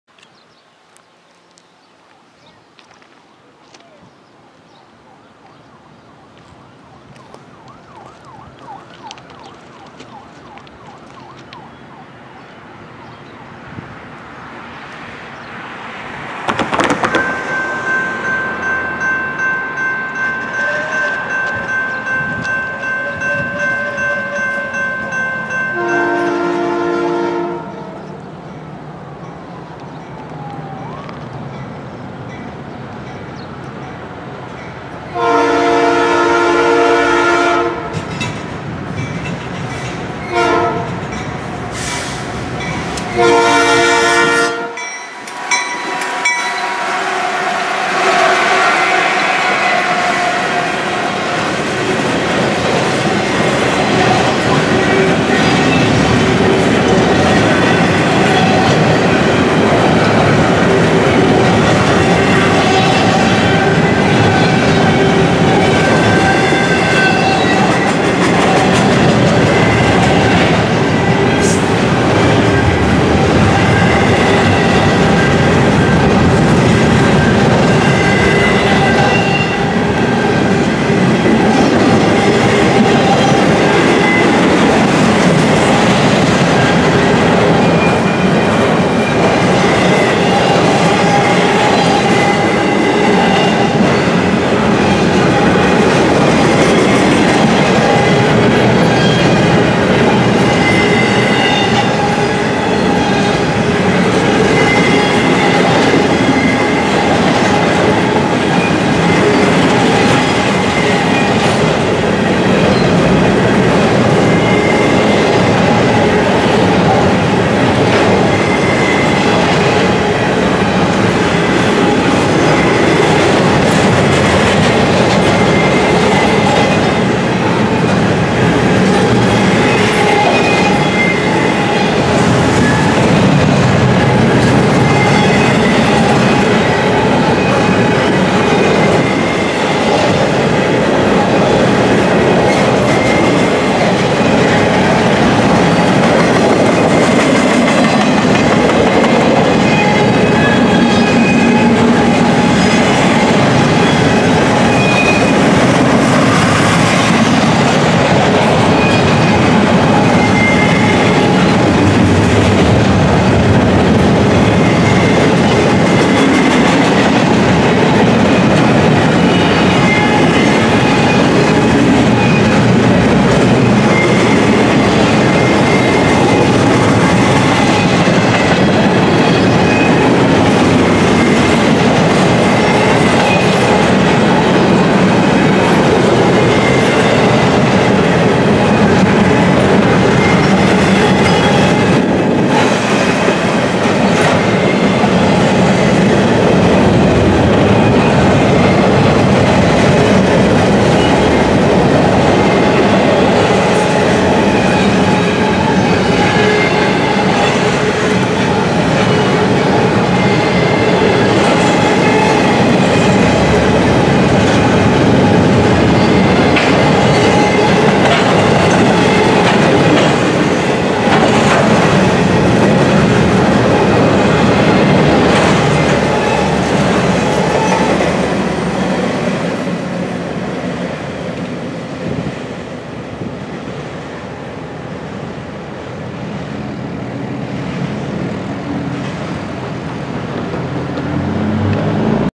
NORFOLK SOUTHERN GE AC44C6M Northbound Intermodal Container Train
A northbound Intermodal Container Lead by Norfolk Southern GE AC44C6M #4030 Engine with a Nathan P5 Horn passing through at Milepost Rosemont.